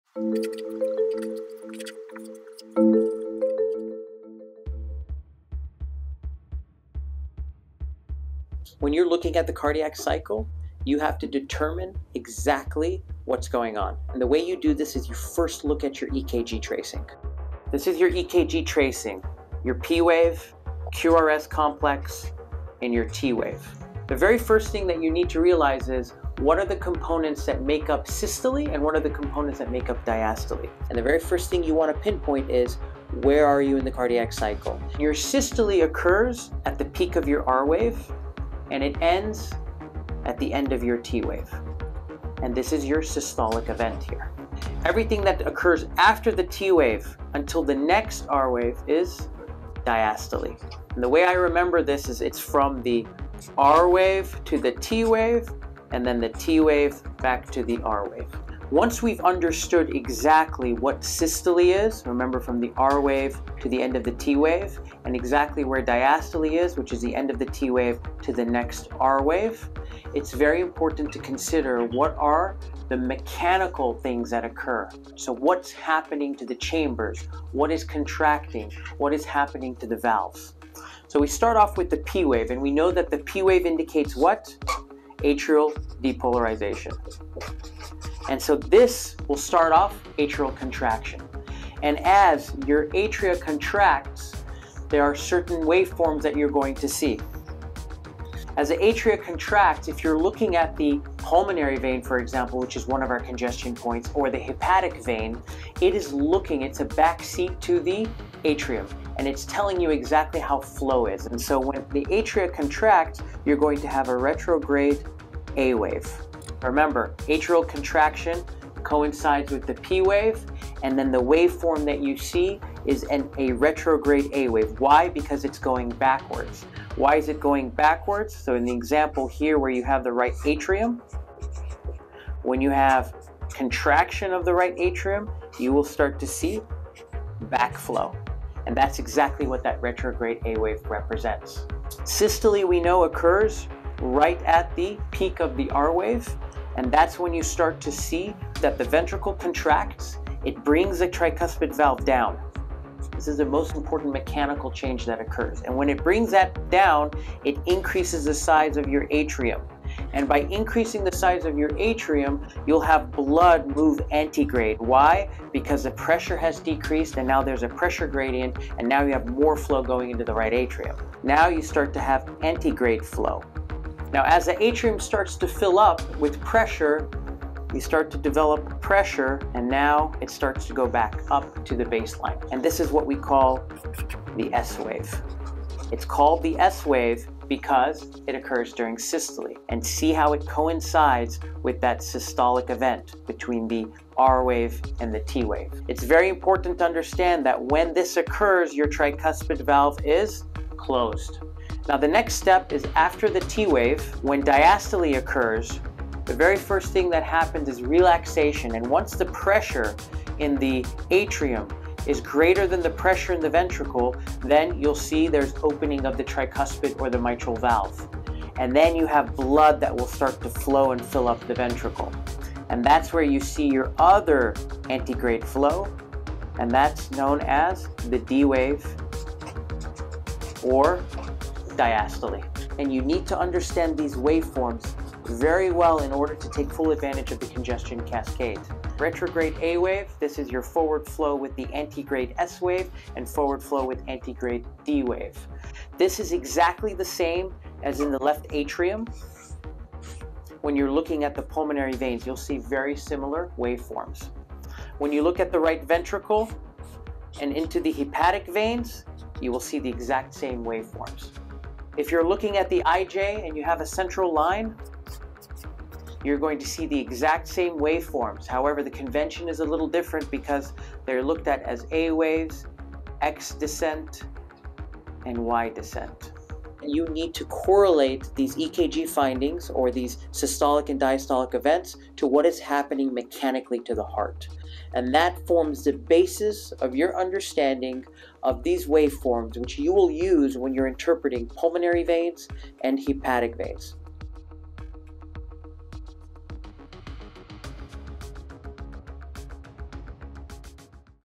How to Understand the Cardiac Cycle and Waveforms - The Pulse Wave Doppler
Before we start our journey through the congestion cascade we need to understand how the wave forms of the cardiac cycle are formed. This video serves as an essential toolkit to develop the skill to understand the POCUS Pulse Wave Doppler of the upcoming chapters of the Congestion Cascade.